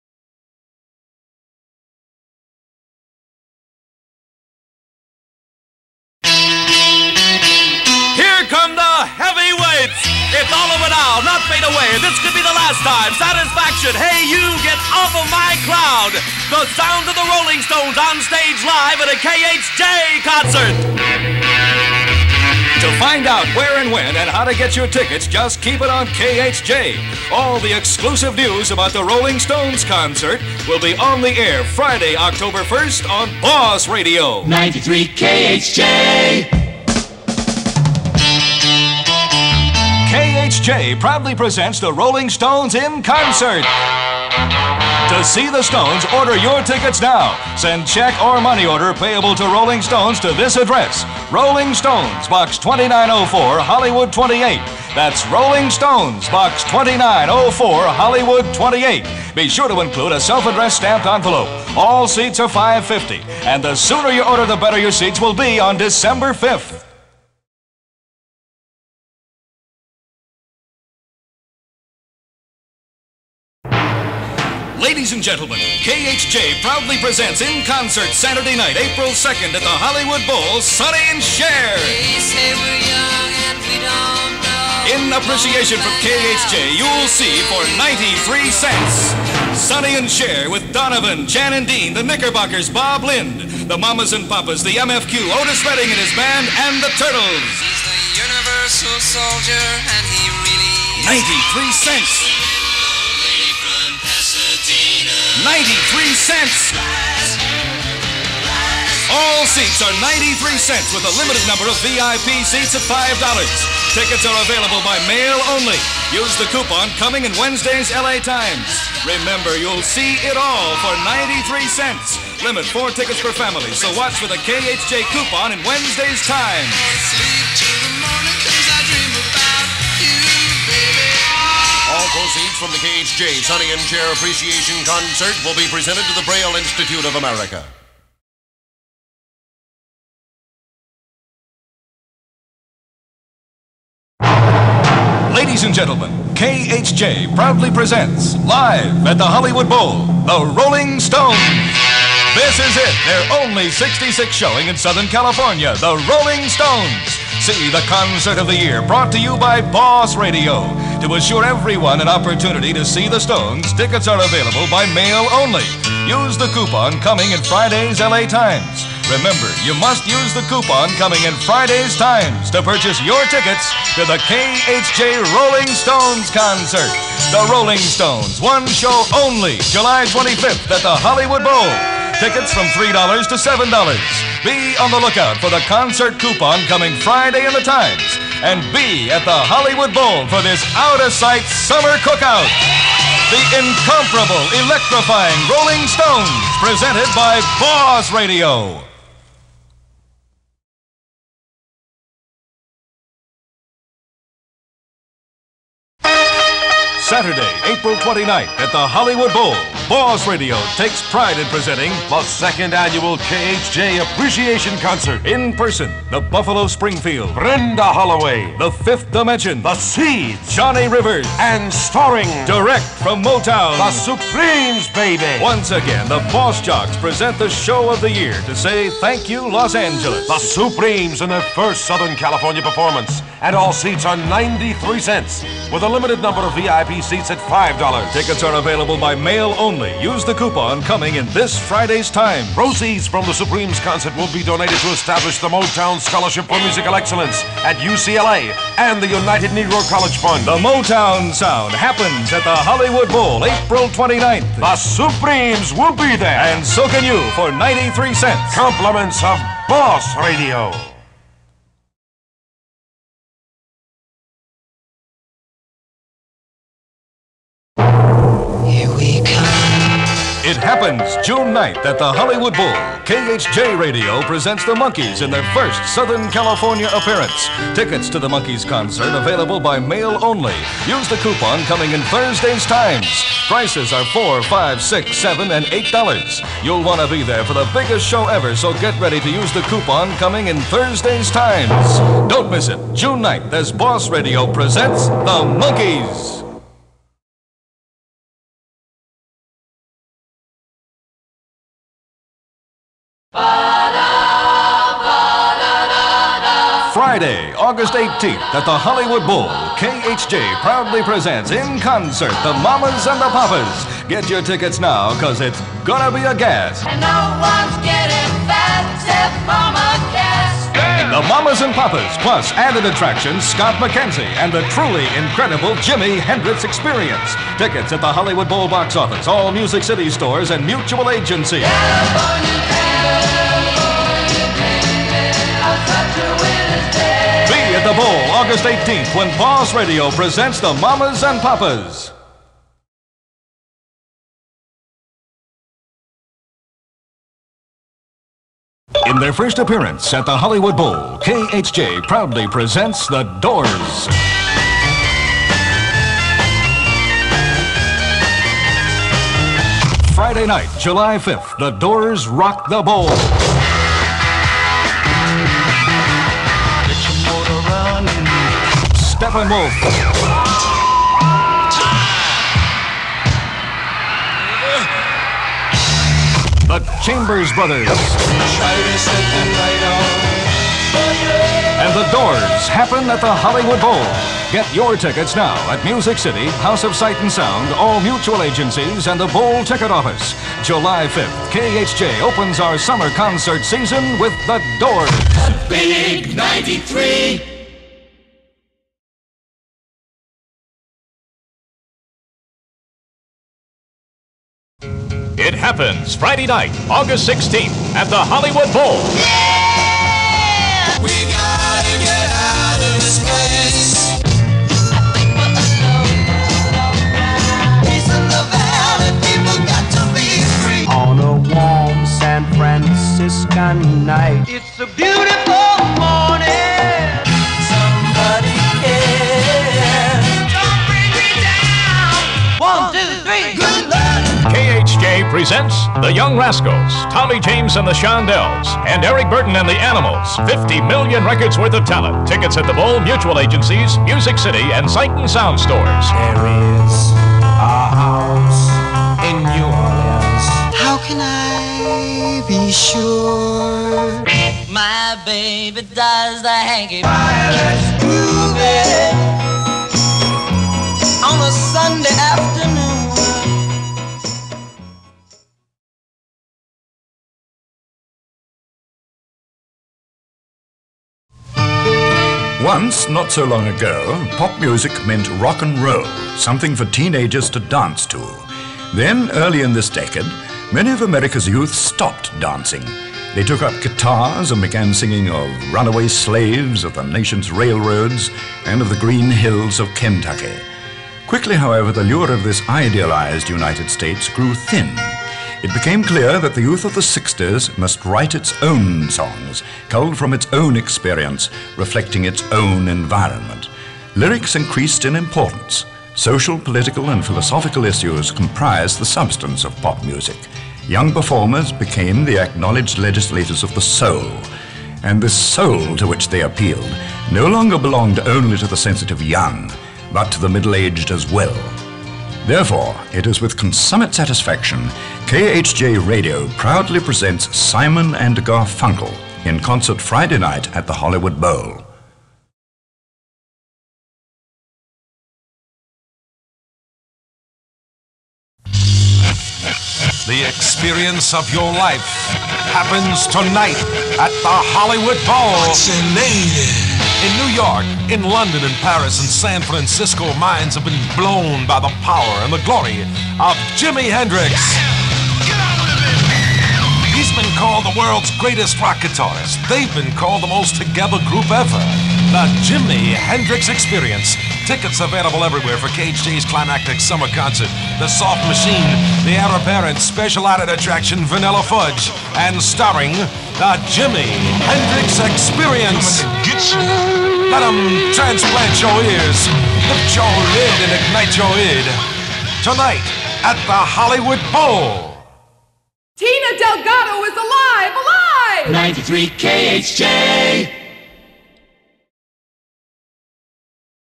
KHJ Boss Radio Concert Promos
The Boss Jocks heard on the spots include Robert W. Morgan, Humble Harve, The Real Don Steele, and Charlie Tuna. Newsman J. Paul Huddleston can be heard at the end of the Sonny & Cher spot. The voice on the Simon & Garfunkel spot is that of actor Maurice Dallimore.